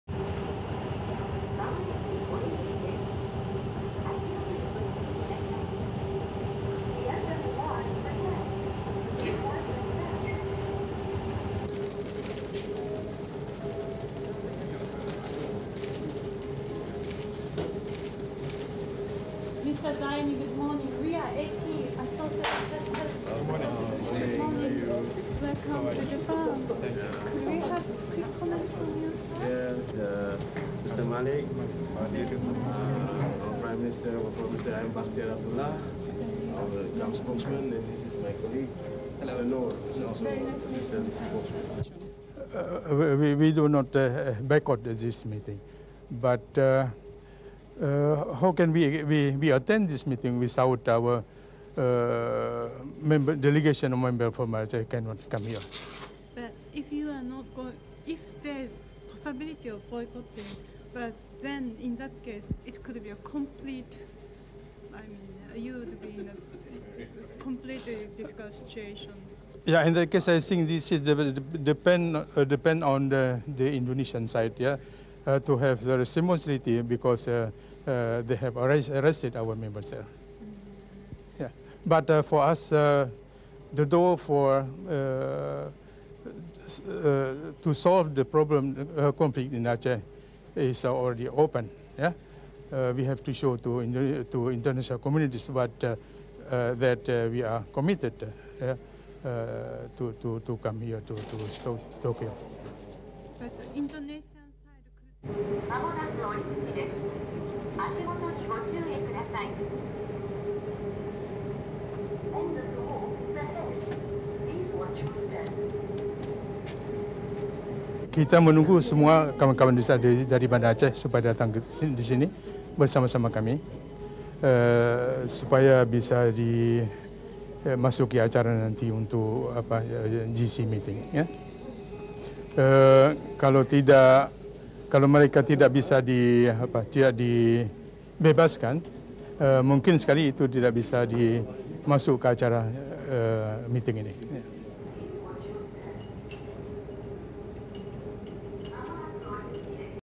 WRAP Aceh negotiator comments as he arrives for peace talks
New Tokyo International Airport, Chiba Prefecture
1. Austrian Airline plane docking
2. Wide shot of Zaini Abdullah, leader of the Free Aceh Movement, walking out together with other delegates
3. Mid shot of Zaini greeting APTN crew
4. SOUNDBITE (English) Zaini Abdullah, chief Aceh negotiator:
"We do not boycott this meeting but how can we attend this meeting without our delegation members from there if they don't come here?"
Q. "But if there is possibility of boycotting, then, in that case, it could lead to the completely difficult situation."
5. SOUNDBITE (English) Zaini Abdullah, chief Aceh negotiator:
"It depends on Indonesian side to have this meeting because they have arrested our members there. But, for us, the door to solve this problem, the conflict in Aceh is already open. We have to show it to the international community that we are committed to come here to Tokyo."
6. Zaini and the delegates walking through terminal
7. Delegates leaving

STORYLINE:

Aceh rebel negotiators, arriving in Tokyo on Saturday for discussions aimed at salvaging a fragile peace deal, said they were ready to meet with the Indonesian government but demanded the release of five detained delegates.

Chief rebel negotiator Zaini Abdullah said the five delegates, who were arrested in the provincial capital of Banda Aceh on Friday as they were departing for Tokyo, must first be freed before the group would come to the negotiating table.

Police said the men were detained because they had not reported to officers before leaving the province - something they are required to do under the peace deal.

The talks aim to save a faltering December 9 peace deal in the oil-and gas-rich province of Aceh that was hailed as a landmark attempt to end a 26-year insurgency which has left 12,000 people dead.

The government insists the rebels must drop their demands for independence, lay down their weapons and accept special autonomy or face a military crackdown. The military has around 30,000 troops in the region against a poorly equipped rebel army estimated to number 3,000 to 5,000.

The Tokyo meeting is taking place after intense pressure from Indonesia's donor countries. The talks were only announced Thursday.